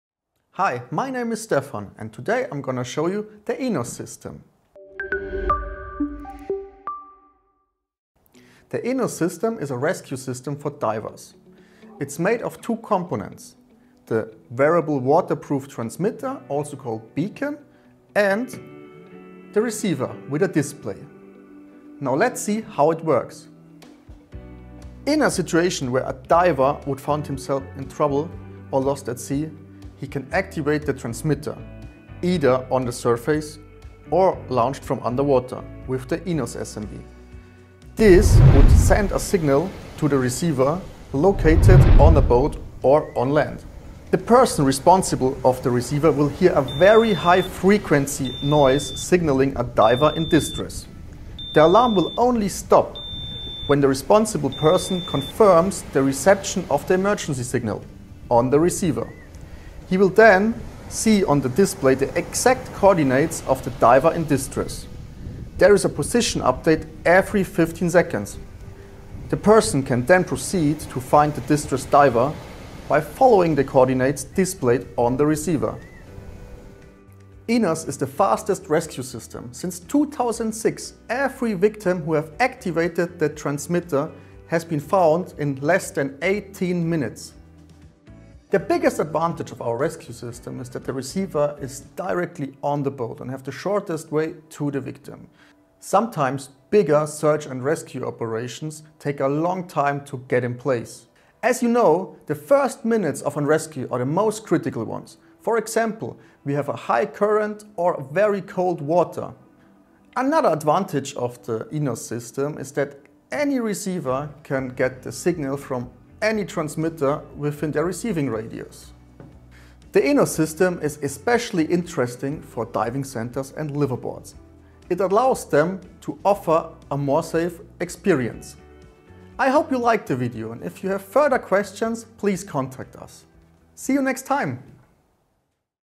What is ENOS rescue system?
In this video Stefan presents the ENOS rescue system.
🇩🇪 100 %  MADE  IN  GERMANY

ENOS is short for “elektronisches Notruf- und Ortungssystem”, it is an electronic rescue and locating system for scuba divers. The diver wears the transmitter on his BCD and the boat has the receiver. If the diver gets lost at sea, he activates the beacon and the boat receives an emergency signal with the exact coordinates of the lost diver!

Worldwide, it is the one and only rescue system for divers showing the crew precisely the positions of the lost divers on its screen.
It functions absolutely independent and autonomously.